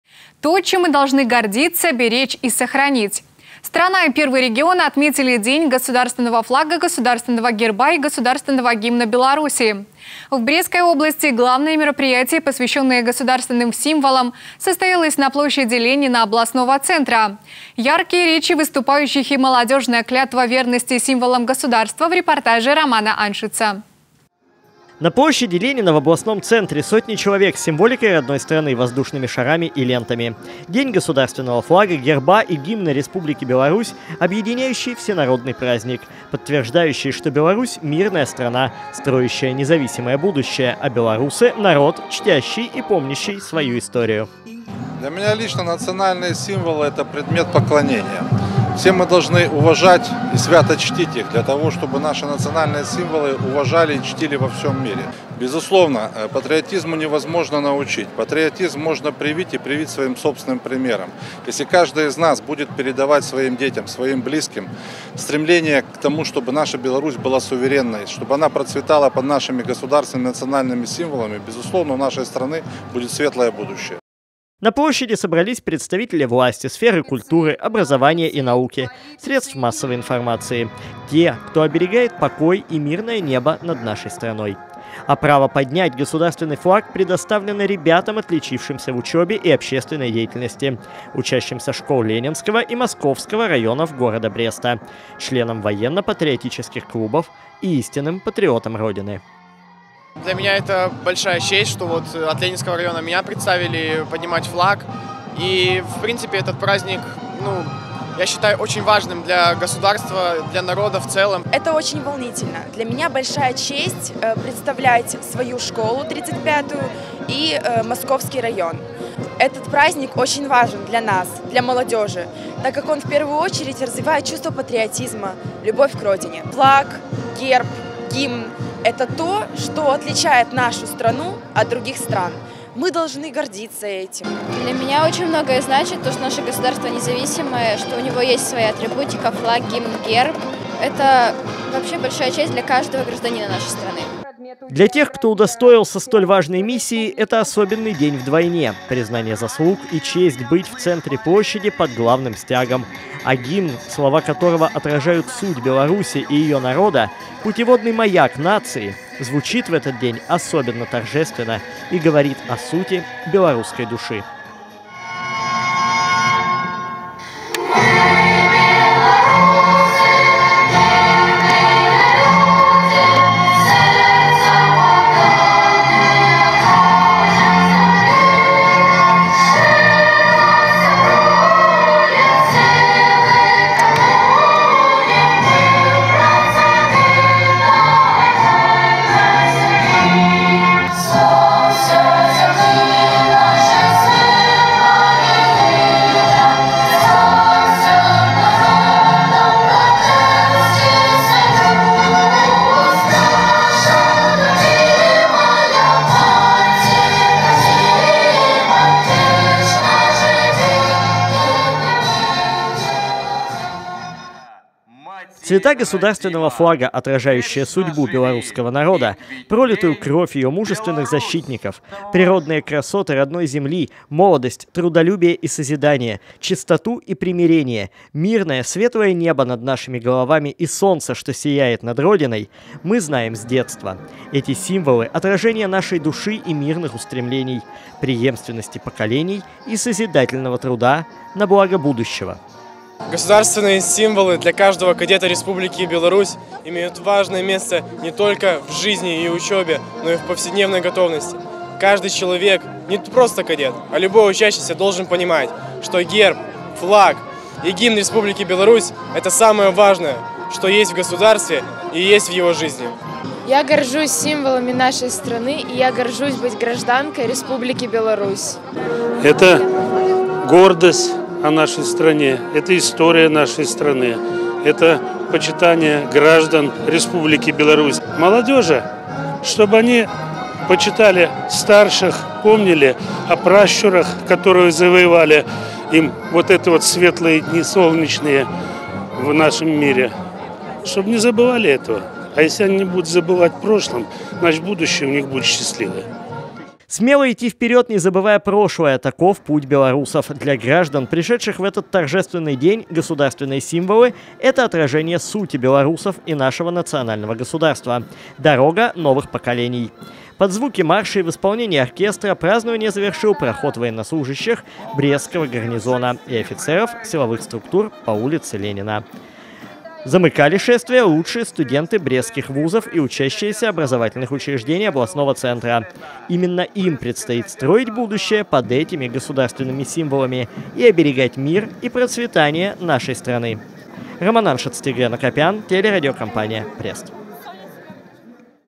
ДЕНЬ ГОСУДАРСТВЕННОГО ФЛАГА, ГЕРБА И ГИМНА
Страна и Первый регион отметили День Государственного флага, Государственного герба и Государственного гимна Беларуси. В Брестской области главное мероприятие, посвящённое государтвенным символам, состоялось на площади Ленина областного центра. Яркие речи выступающих и молодёжная клятва верности символам государства - в репортаже Романа Аншица.